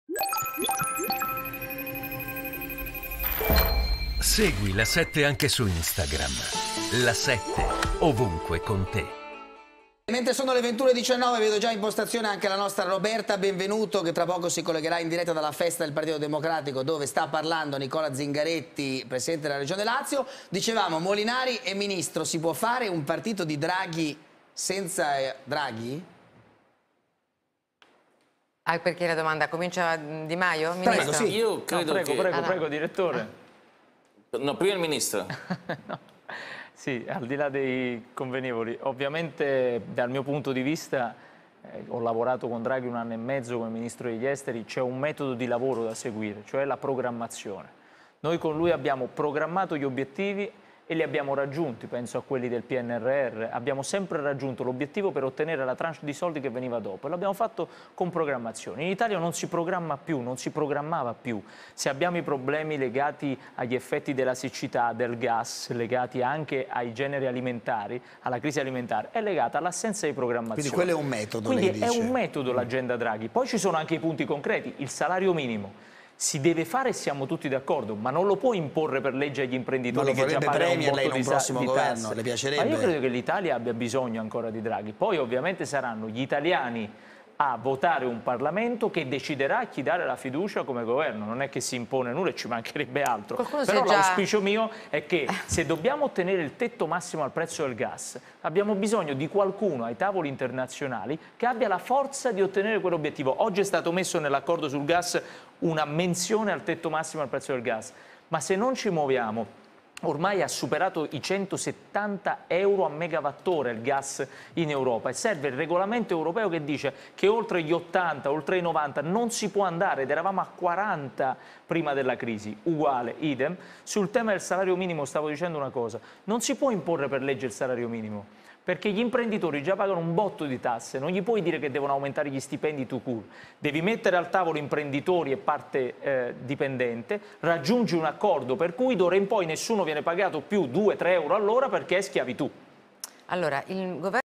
Luigi Di Maio: "L'Italia ha ancora bisogno di Draghi"
Luigi Di Maio: "Draghi di nuovo premier? Credo che l'Italia abbia ancora bisogno di Draghi"